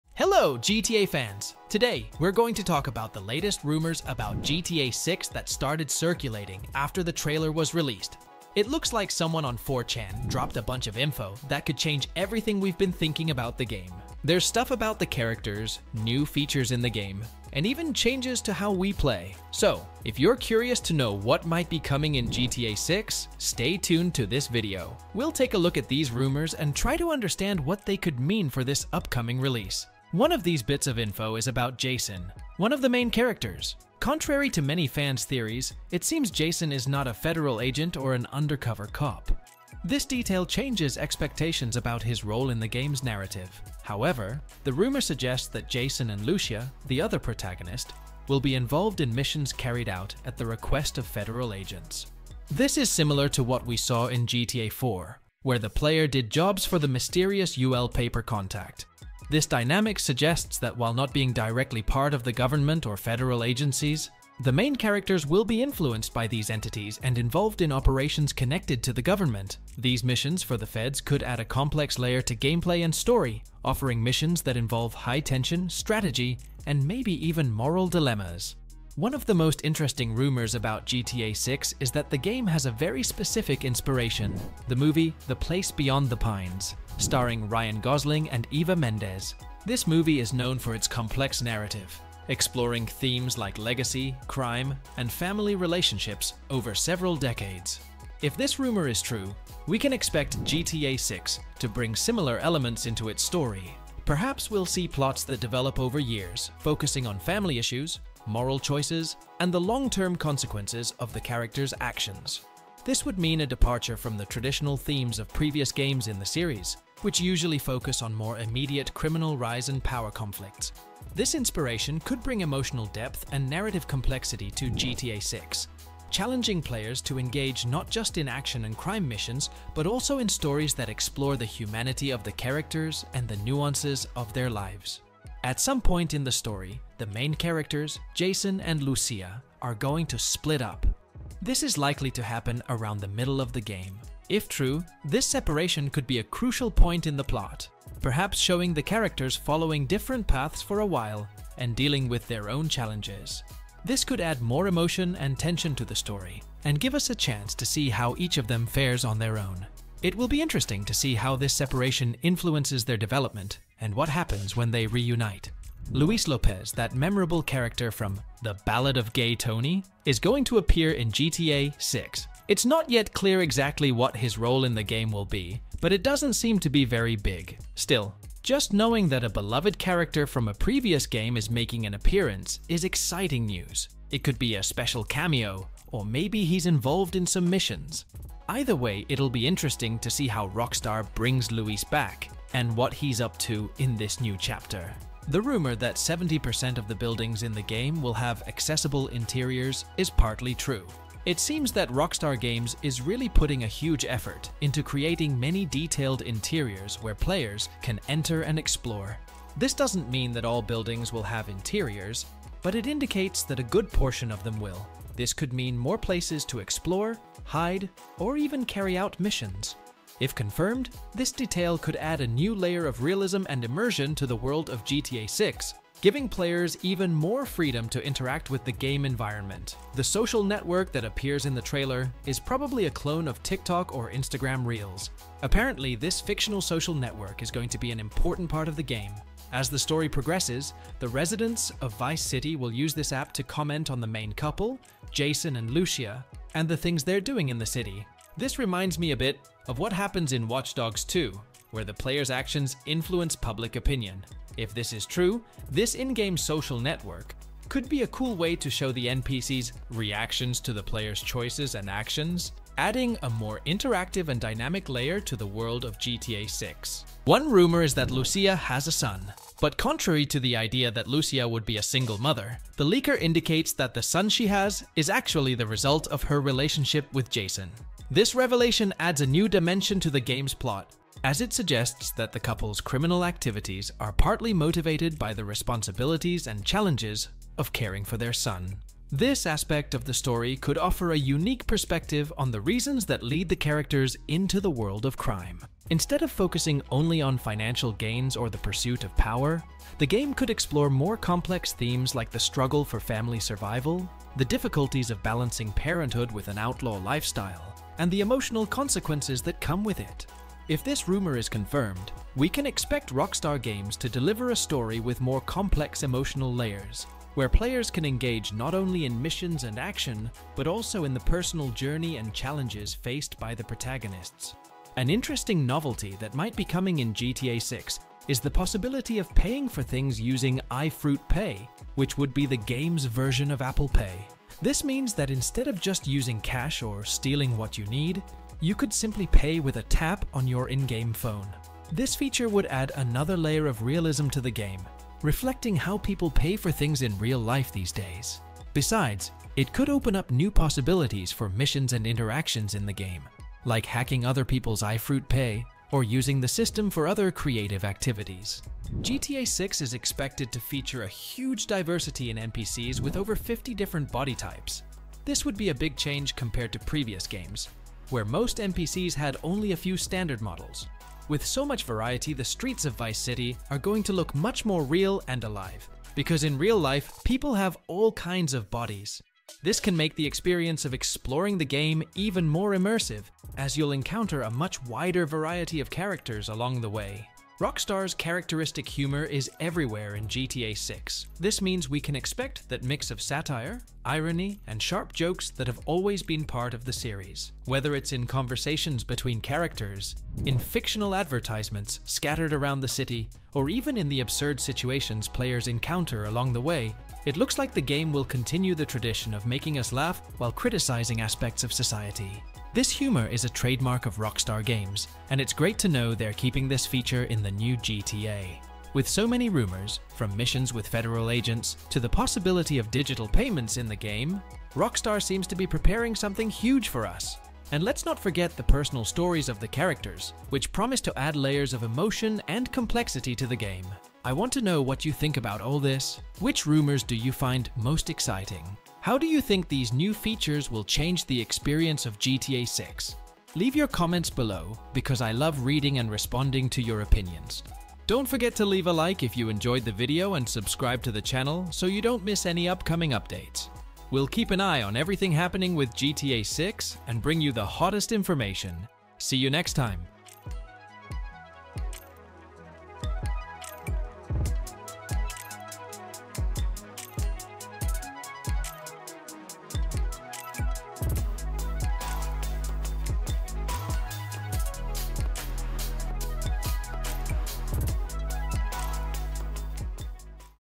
GTA 6 Has New LEAKED Details! (Jason Unmasked, Old Character AND More!)
In the video, we delve into the latest GTA 6 rumors following its thrilling trailer release. From unexpected revelations about character Jason to the game's cinematic inspiration and the return of a familiar face from 'The Ballad of Gay Tony', we cover it all. We also explore new in-game features like detailed building interiors, a digital payment system, diverse NPCs, and Rockstar's signature humor. Join us for a concise yet exciting look at what might be in store for GTA 6.